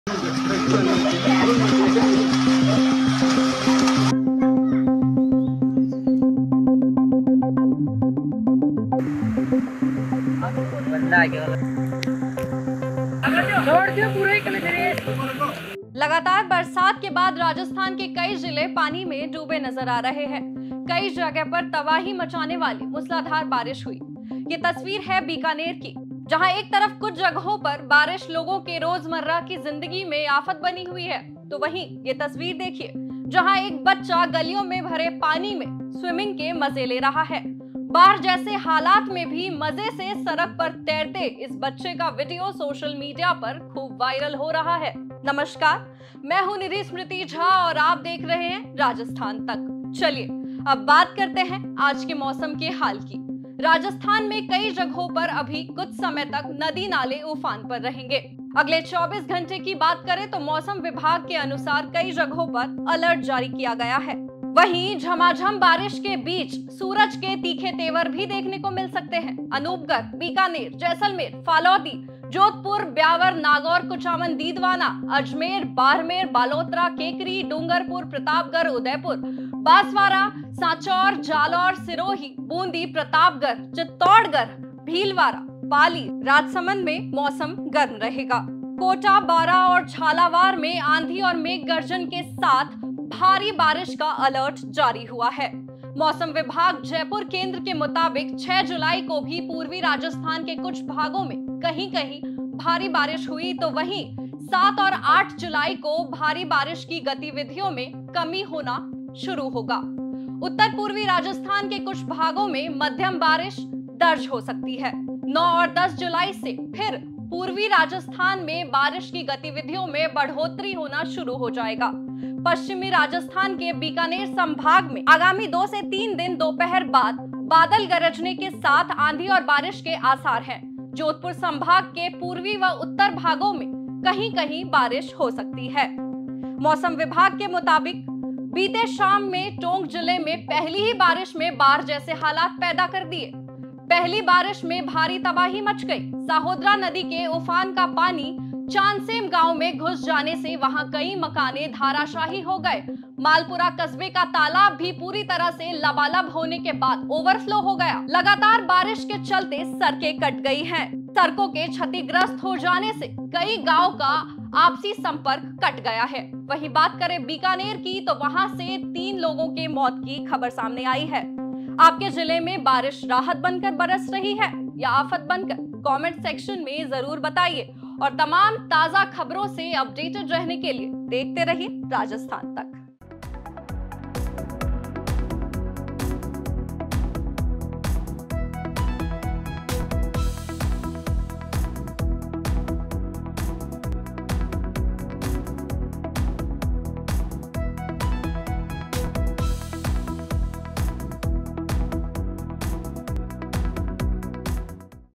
Rajasthan Weather update आपदा को बनाया अवसर! जानिए क्या रहेगा मौसम का हाल| Heavy Rain| Weather Today
लगातार बरसात के बाद राजस्थान के कई जि़ले पानी में डूबे नज़र आ रहे हैं। कई जगह पर तबाही मचाने वाली मुसलाधार बारिश हुई। जहां एक तरफ कुछ जगहों पर बारिश लोगों के रोज़मर्रा की जिंदगी में आफत बनी हुई है तो वहीं एक बच्चा गलियों में भरे पानी में स्विमिंग के मज़े ले रहा है। बाढ़ जैसे हालात में भी मज़े से सड़क पर तैरते इस बच्चे का वीडियो सोशल मीडिया पर खूब वायरल हो रहा है। राजस्थान में कई जगहों पर अभी कुछ समय तक नदी-नाले उफान पर रहेंगे। अगले 24 घंटे की बात करें तो मौसम विभाग के अनुसार कई जगहों पर अलर्ट जारी किया गया है। वहीं, झमाझम बारिश के बीच सुरज के तीखे तेवर भी देखने को मिल सकते हैं।